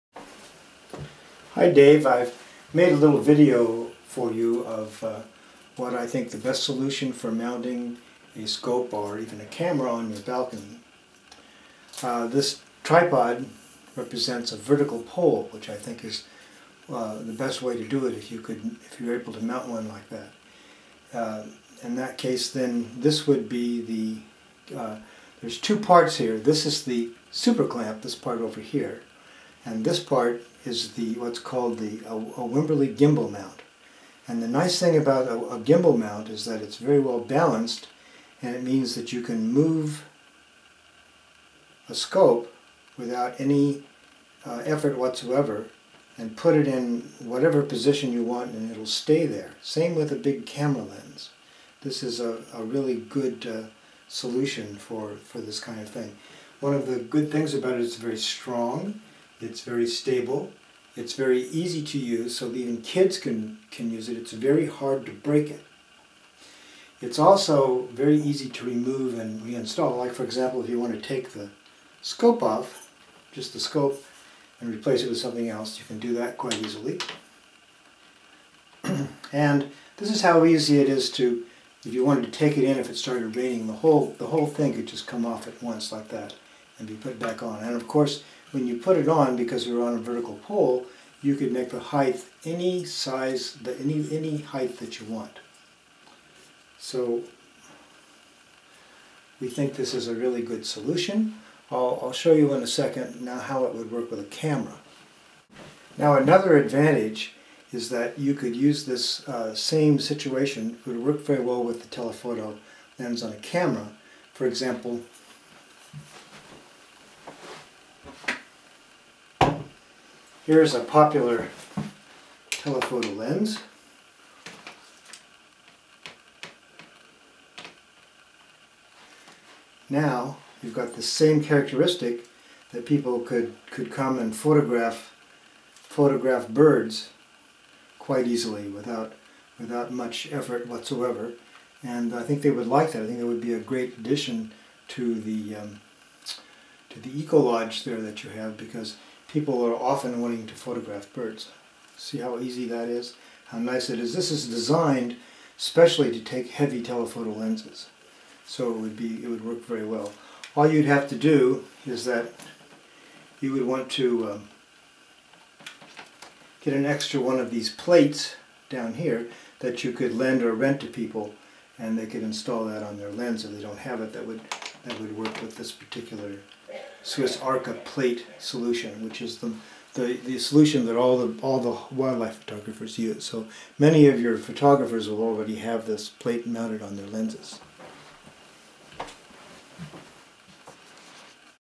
Dave Hayles Mount Solution
A way to mount a scope or camera on your balcony.  Easy to install and remove.